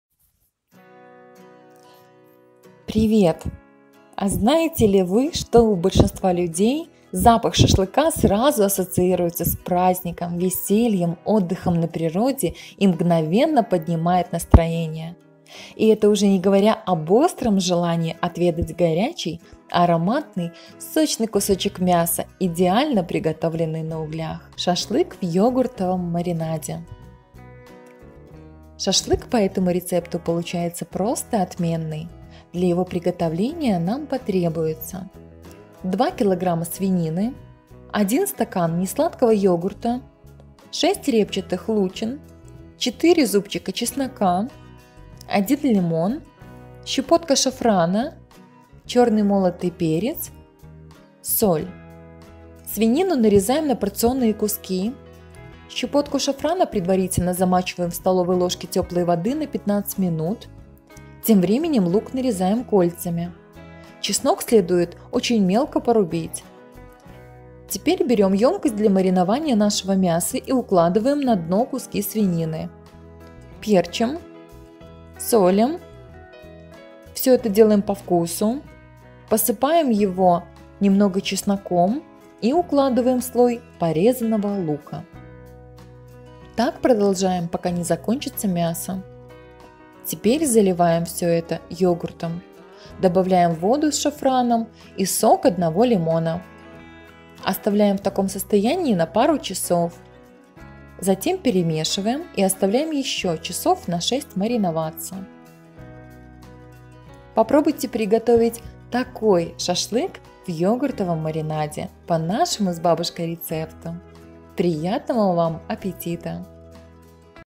ШАШЛЫК В ЙОГУРТОВОМ МАРИНАДЕ. КАК ЗАМАРИНОВАТЬ ШАШЛЫК
Шашлык по этому рецепту получается просто отменный. Для его приготовления нам потребуется:
* 2 кг свинины
* 1 стакан не сладкого йогурта
* 6 репчатых лучин
* 4 зубка чеснока
* 1 лимон
* щепотка шафрана
* черный молотый перец
* соль
Свинину нарезаем на порционные куски. Щепотку шафрана предварительно замачиваем в столовой ложке теплой воды на 15 минут. Тем временем лук нарезаем кольцами. Чеснок следует очень мелко порубить. Теперь берем емкость для маринования нашего мяса и укладываем на дно куски свинины, перчим, солим его по вкусу, посыпаем немного чесноком и укладываем слой порезанного лука. Так продолжаем пока не закончится мясо. Теперь заливаем все это йогуртом, добавляем воду с шафраном и сок одного лимона. Оставляем в таком состоянии на пару часов, затем перемешиваем и оставляем еще часов на 6 мариноваться.